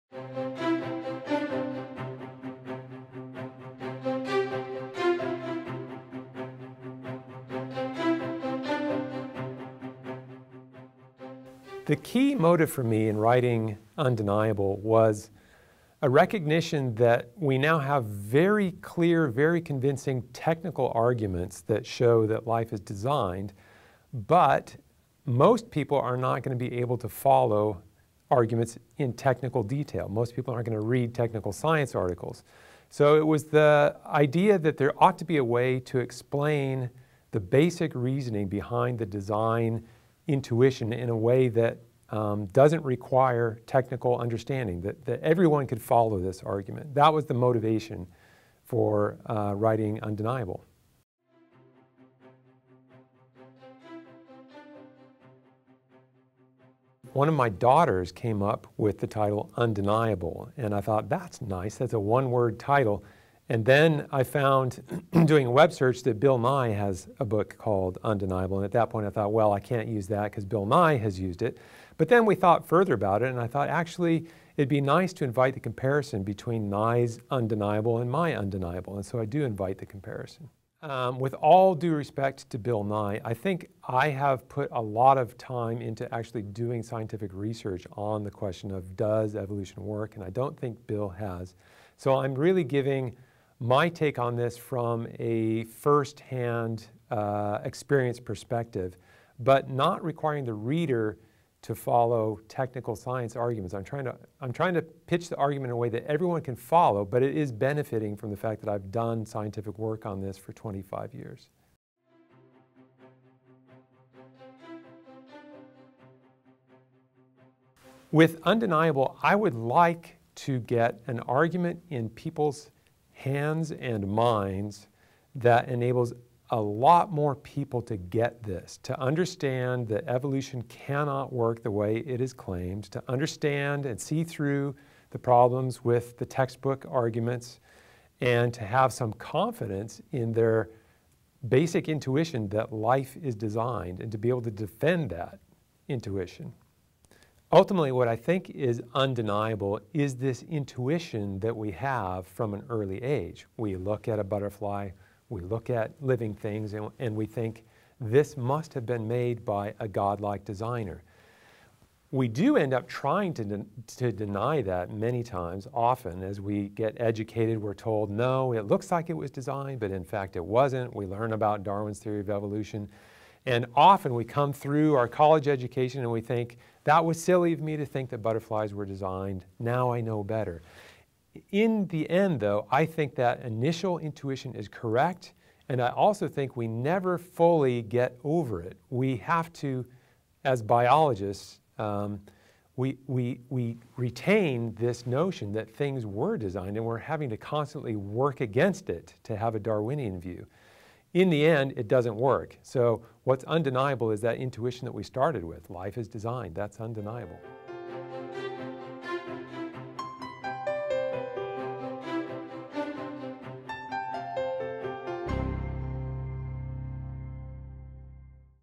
Conversations with Douglas Axe: Why Did You Write Undeniable?
Biologist Doug Axe is the author of Undeniable: How Biology Confirms Our Intuition That Life Is Designed (HarperOne 2016, In Undeniable Doug Axe restores the place of intuition alongside intellect in considering the question of life's origins. In Undeniable, Axe dismantles the widespread belief, that Darwin’s theory of evolution is indisputably true, showing instead that a gaping hole has been at its center from the beginning.